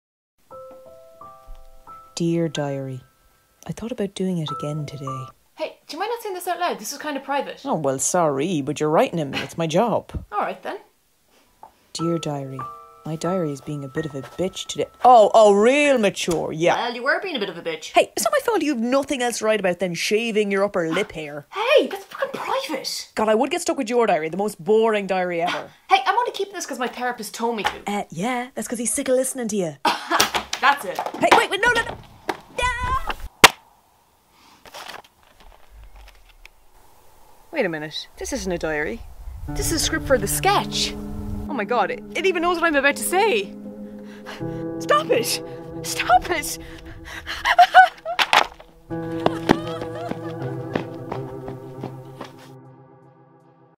Dear Diary - A Very Short Film
You can call me Meta Thunberg or Meta Cheese after watching this. I’ve no idea wtf this is. A short film I made today. Self shooting is hard sometimes I tried to do a focus pull while writing at the same time but alas it did not work. Enjoy (?)